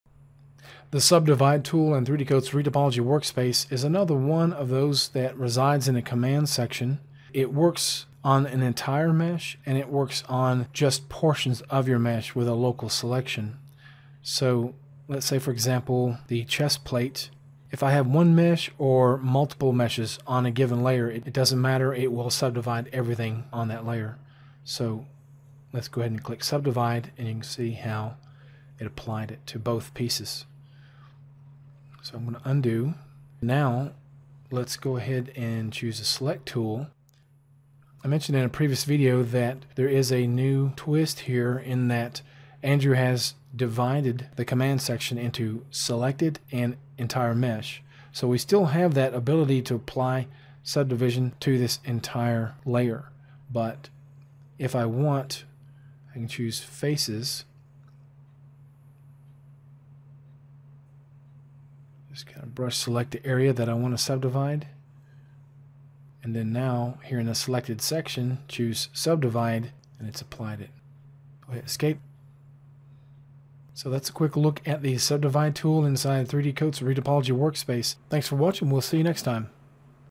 Subdivide Tool
This video demonstrates the usage of the Subdivide tool in the Retopo Workspace of 3D Coat